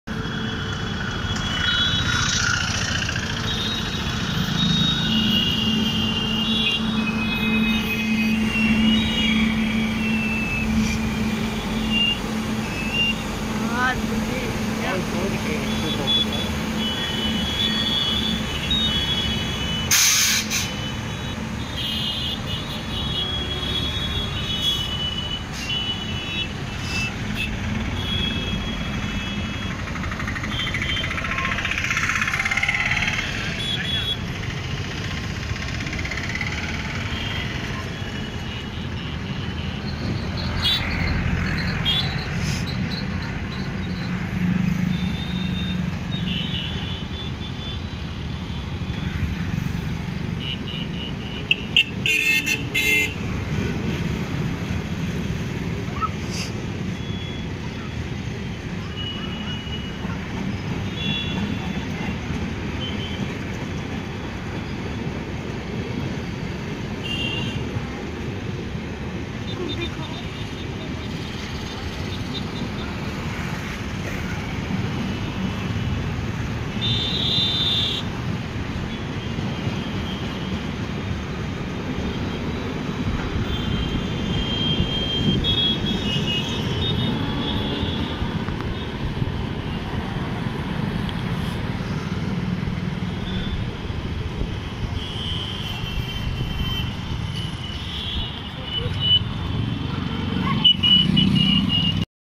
19039/अवध एक्सप्रेस को सिंग्नल मिलते ही गोरखपुर जंक्शन की तरफ तेजी से बढ़ती हुई
The train is 19039/Bandra Terminus - Muzaffarpur Avadh Express

AvadhExprss_ gorakhpur junction